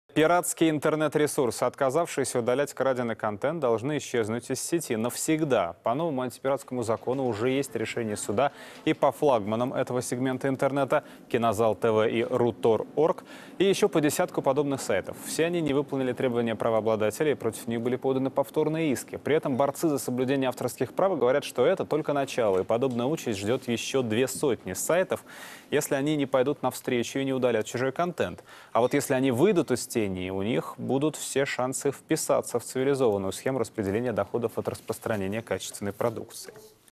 Пиратские интернет-ресурсы, отказавшиеся удалять краденый контент, должны исчезнуть из Сети навсегда
По новому антипиратскому закону уже есть решение суда и по флагманам этого сегмента Интернета - Кинозал.тв и Рутор.орг, и ещё по десятку подобных сайтов. Все они не выполнили требования правообладателей, и против них были поданы повторные иски.